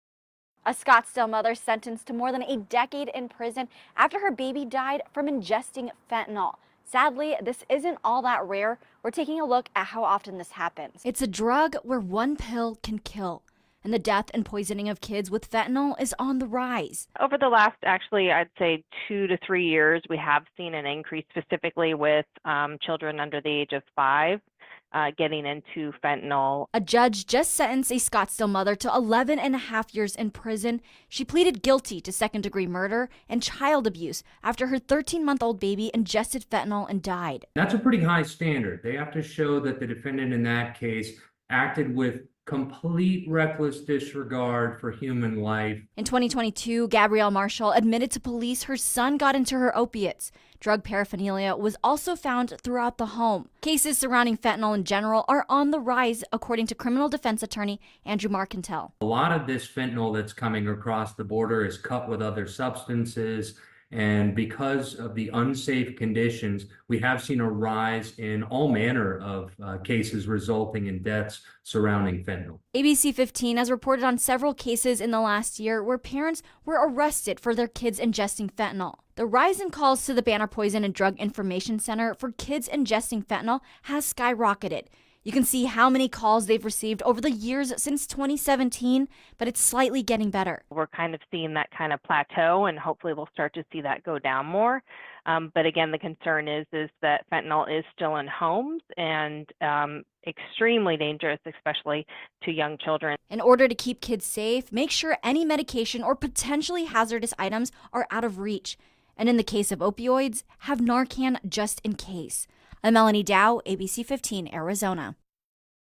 Mother sentenced to 11 years in prison after guilty plea in her child's overdose death
A Scottsdale mom will serve more than a decade behind bars after she pleaded guilty to second-degree murder in the overdose death of her 13-month-old child.  In September of 2022, Scottsdale police received a call regarding a young child who had ingested fentanyl.